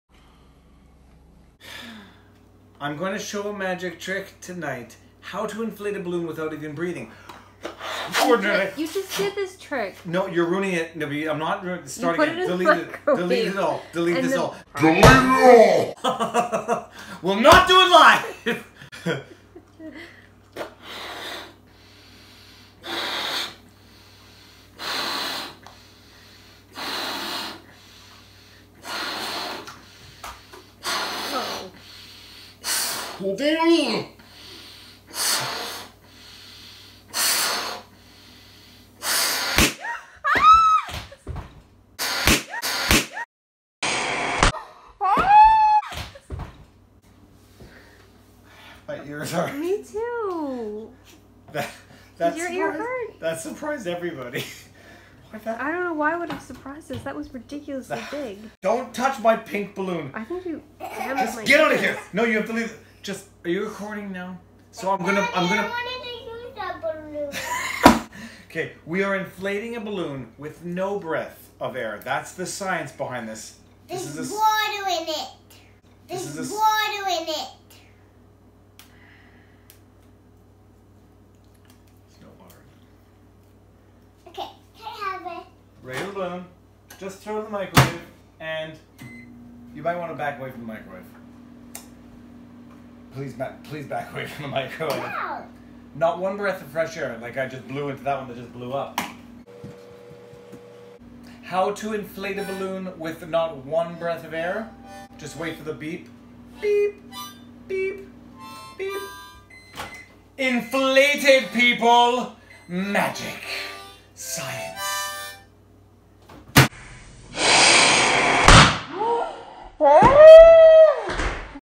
HOW TO INFLATE A BALOON WITHOUT BLOWING!!!
Just some random dad science - how to inflate a baloon without using your breath. Warning: It gets hot! And loud! Peace out!